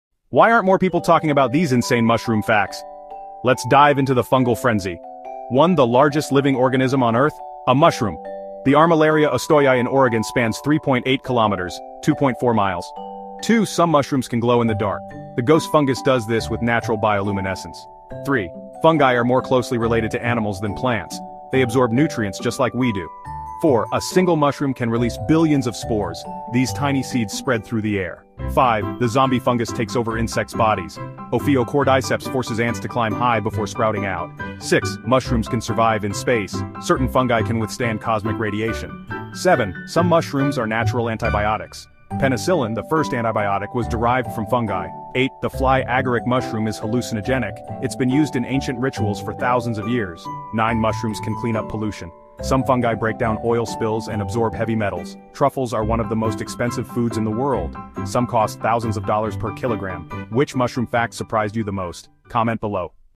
🍄 10 Incredible Mushroom Facts You Won’t Believe! 🍄
Mushrooms are more than just food—they’re **nature’s secret wonders!** Did you know that the **largest living organism** on Earth is a mushroom covering **3.8 km**? 🌍🍄 Some mushrooms even **glow in the dark**, while others can **survive in space**! 🚀✨ There’s even a **Zombie Fungus** that controls insects' minds! 🧟‍♂️🐜